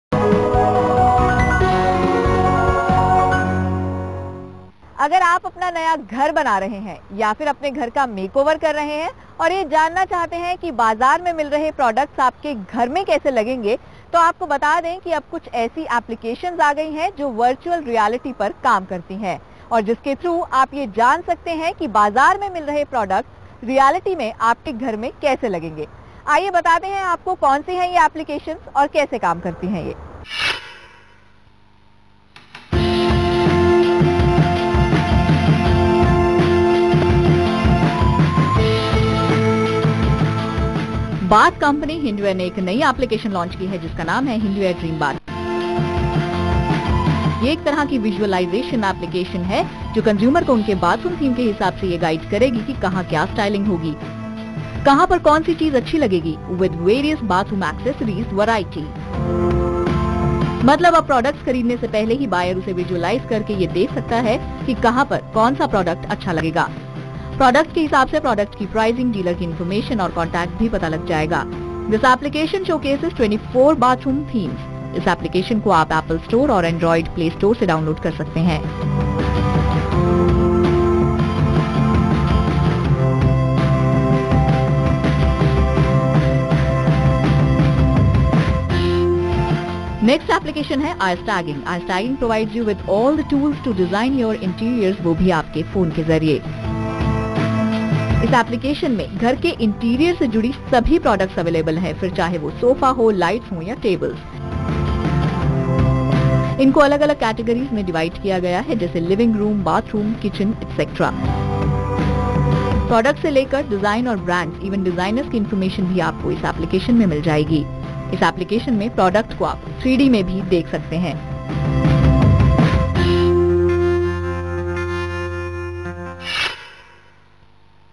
Zee Business features Hindware DreamBath App
Dreambath app gets featured on zee business.
- you can use dreambath app to design your bathrooms in virtual reality.
- you can also watch how the products look in your bathroom with the augmented reality feature.